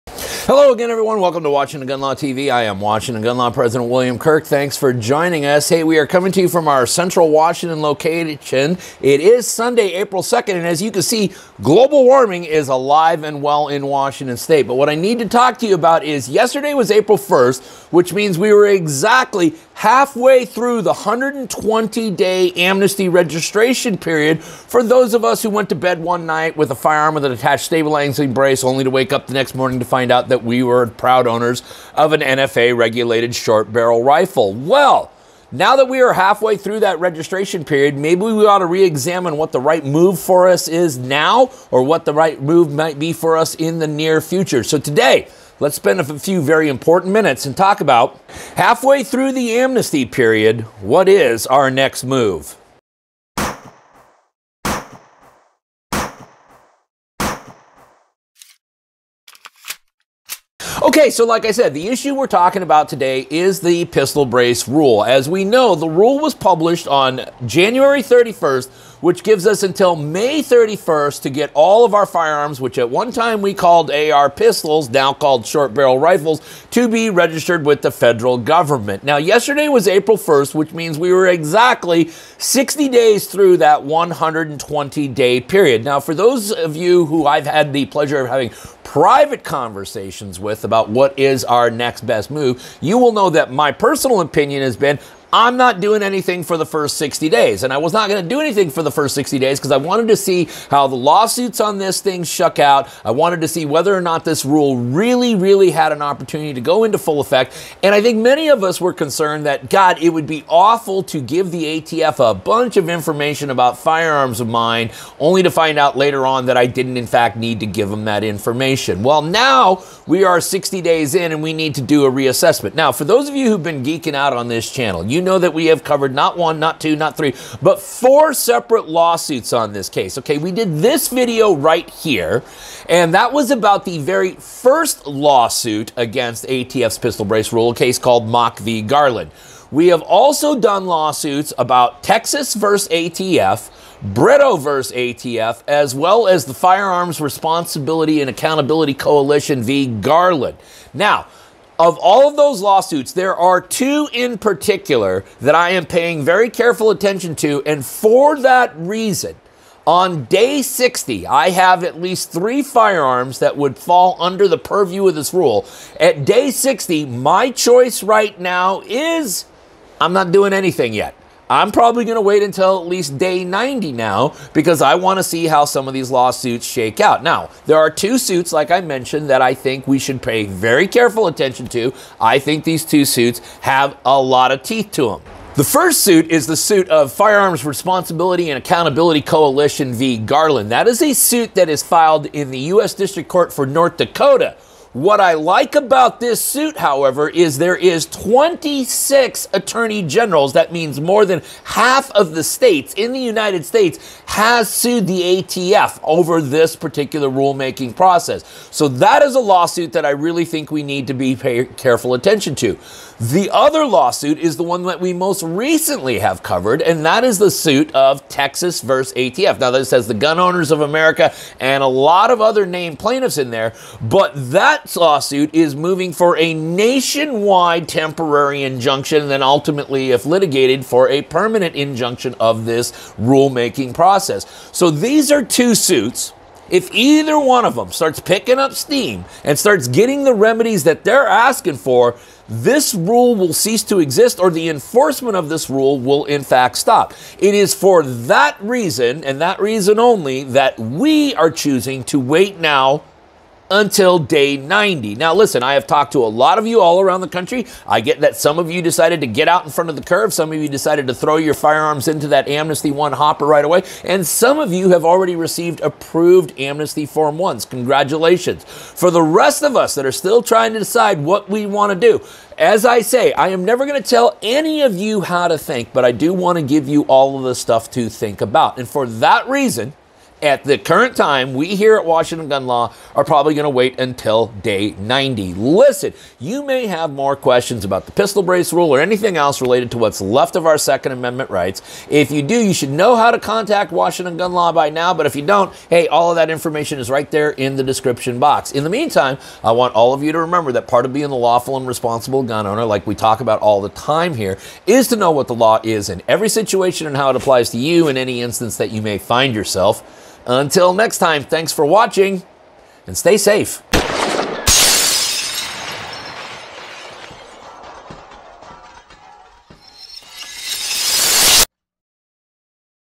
Halfway Through the Amnesty Period, What Is Our Next Move?
Washington Gun Law President, William Kirk, marks the 60 day mark of the 120 day "amnesty period" where we can register our once AR pistol, now SBRs, under a unique Form 1 process.  The rule, which was published on January 31, 2023 means that we have until May 31, 2023 to be in compliance with this rule...if the rule survives that long.  So what is our next best move?   Learn what all of this means to you and arm yourself with education.
Applicable Case.

Stay safe.

How the Supreme Court Just Boxed in the EPA, and What That Means For the ATF.